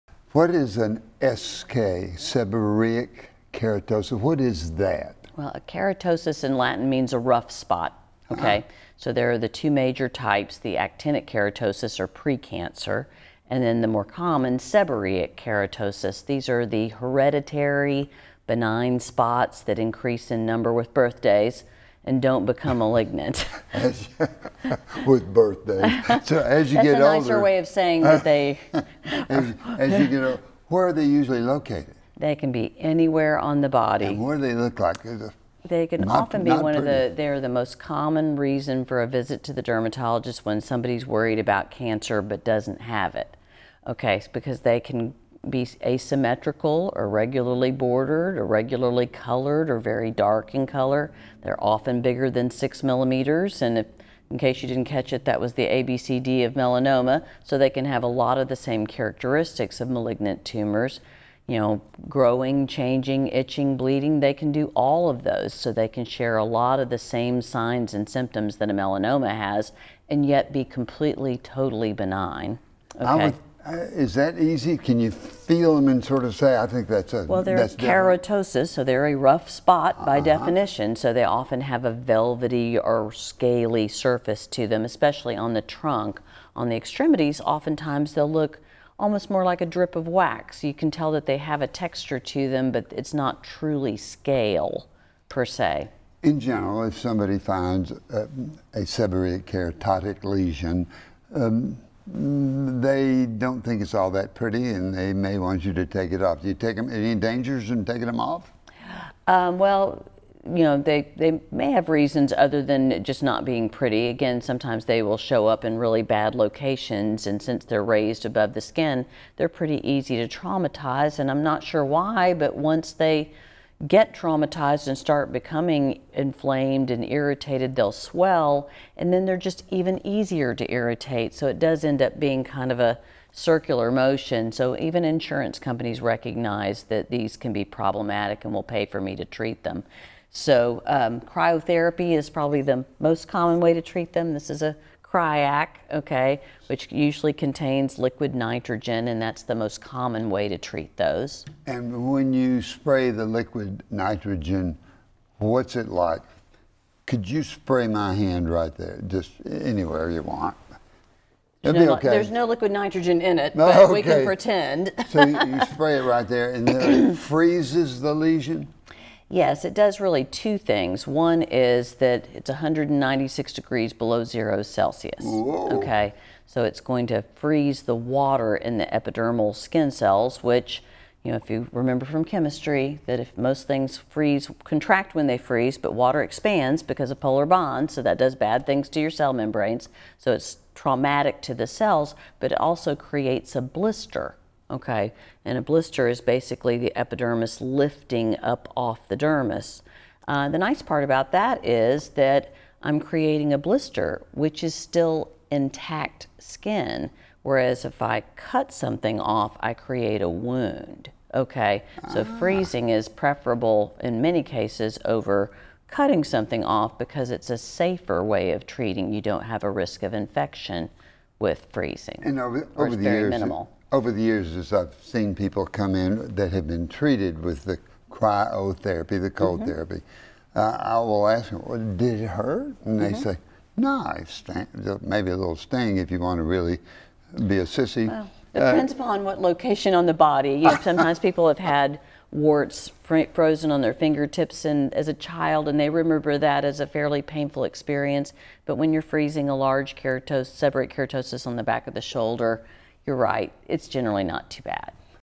Seborrheic Keratosis
Dr. Meredith Overholt, dermatologist,  discusses seborrheic keratosis -- a common and noncancerous skin growth.